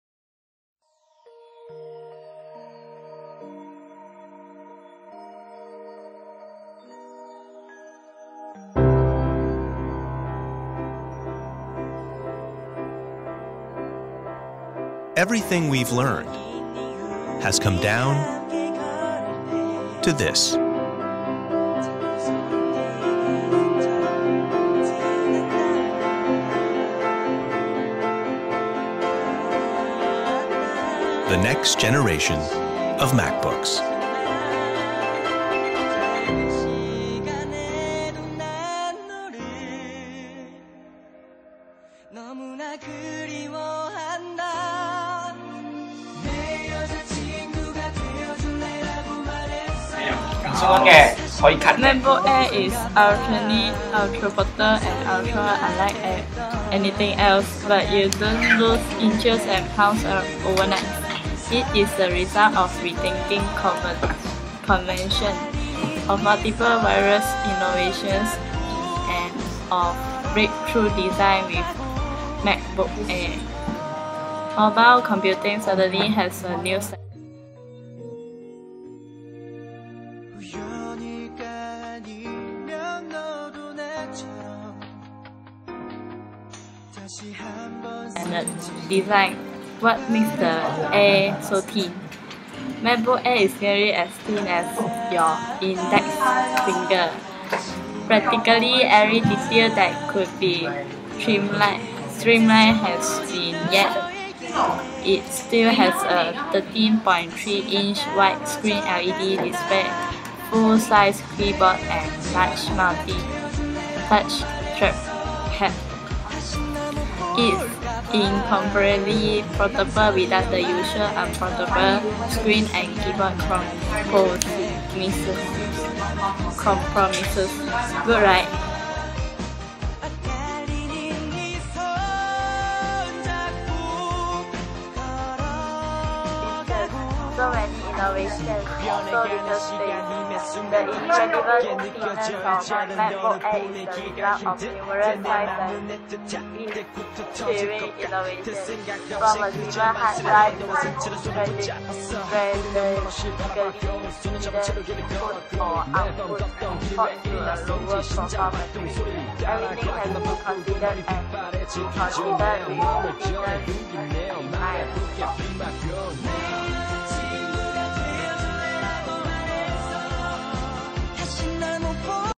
RE2 - Anne Mah
MY FIRST IMOVIE PROJECT, INTRODUCTION OF MACBOOK AIR 2012